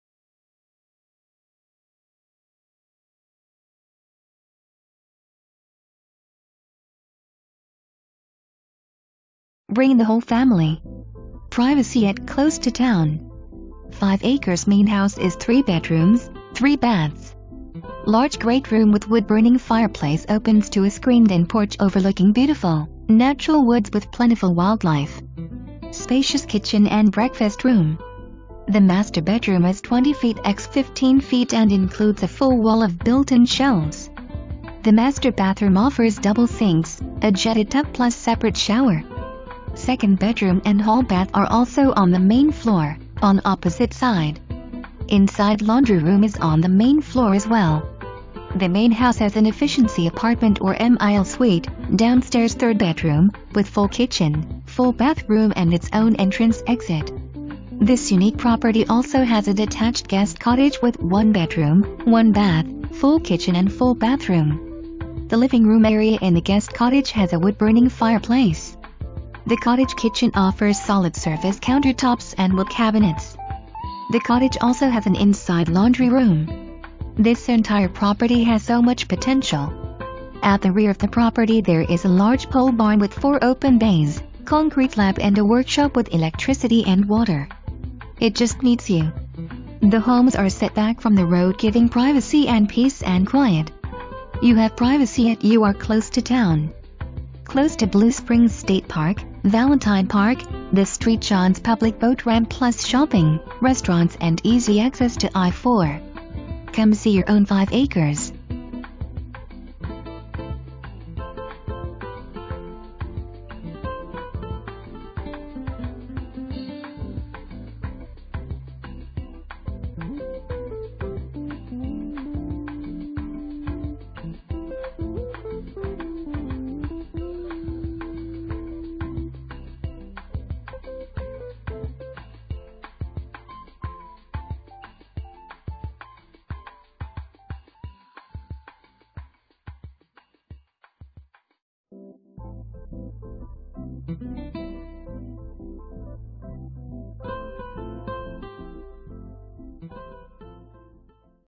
555 WERLEY TRAIL
Bring the whole family!! Privacy yet close to town! 5 ACRES. Main house is 3 bedrooms, 3 baths. Large Great Room with wood-burning fireplace opens to a screened in porch overlooking beautiful, natural woods with plentiful wildlife. Spacious kitchen and breakfast room. The master bedroom is 20' x 15' and includes a full wall of built in shelves. The master bathroom offers double sinks, a jetted tub plus separate shower. Second bedroom and hall bath are also on the main floor, on opposite side. Inside laundry room is on the main floor as well. The main house has an efficiency apartment or MIL suite (downstairs 3rd bedroom) with full kitchen, full bathroom and its own entrance/exit. This unique property also has a detached guest cottage with 1 bedroom, 1 bath, full kitchen and full bathroom. The living room area in the guest cottage has a wood burning fireplace. The cottage kitchen offers solid surface counter tops and wood cabinets. The cottage also has an inside laundry room. This entire property has so much potential. At the rear of the property there is a large pole barn with 4 open bays, concrete slab and a workshop with electricity and water!  It just needs you! The homes are set back from the road giving privacy and peace and quiet. You have privacy yet you are close to town! Close to Blue Springs State Park, Valentine Park, the St Johns public boat ramp plus shopping, restaurants and easy access to I-4. Come see your own 5 acres!

Presented By:
Patricia Ramsby, Berkshire Hathaway HomeServices Florida Realty
View My Inventory: